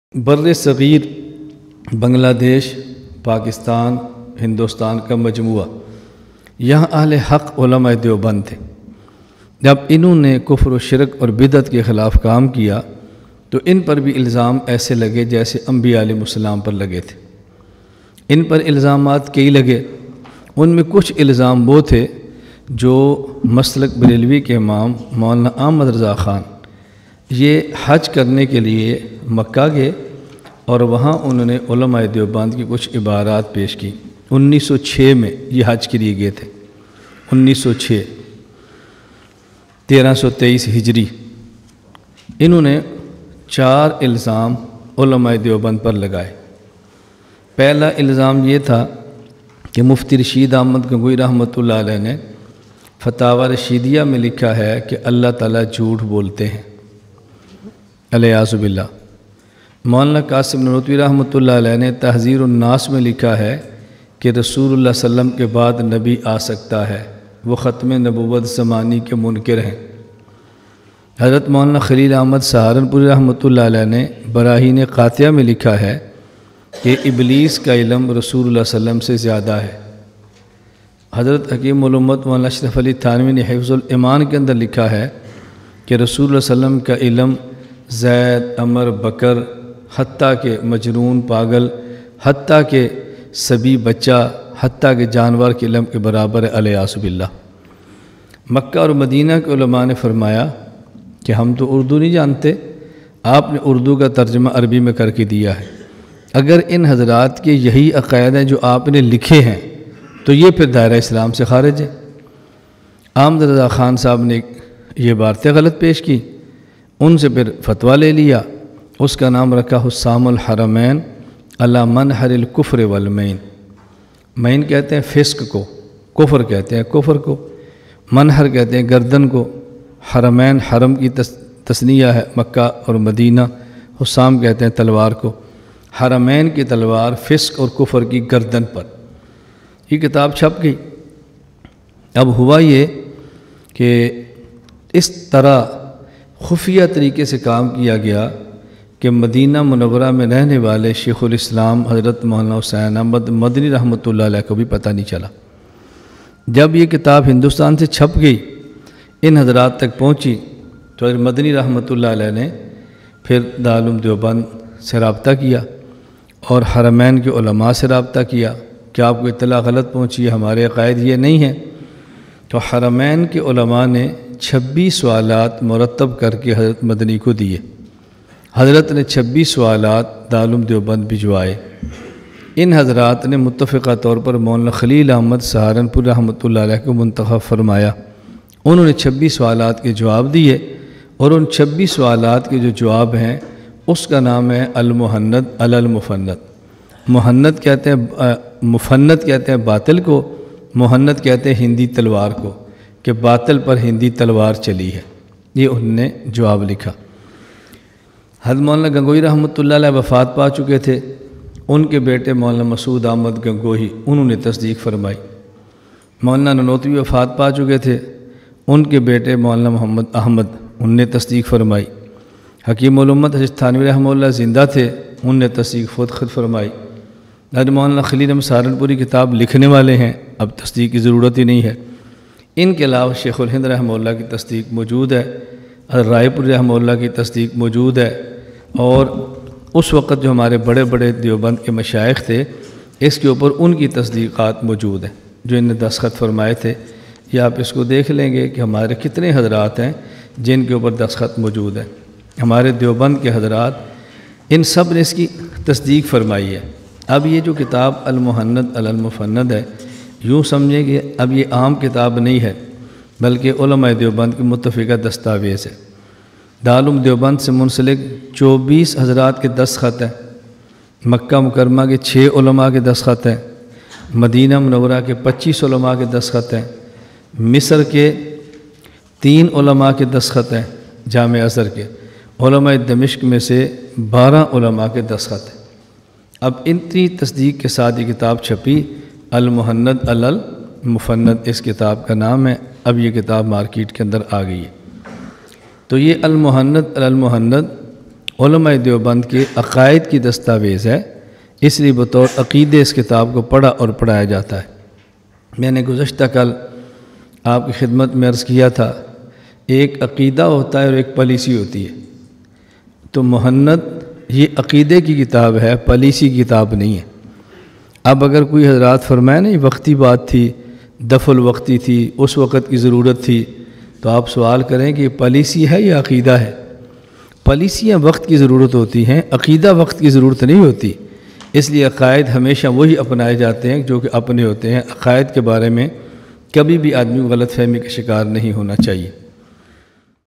Ahmad Raza Khan Ky Ilzamat Ki Haqiqat Or Ulma Deoband Ka Difa | الزامات کی حقیقت
متکلم اسلام وکیل احناف شیخ طریقت مولانا محمد الیاس گھمن دامت برکاتہم العالیہ
سرپرست اعلی مرکز اہل السنۃ والجماعۃ و خانقاہ حنفیہ سرگودہا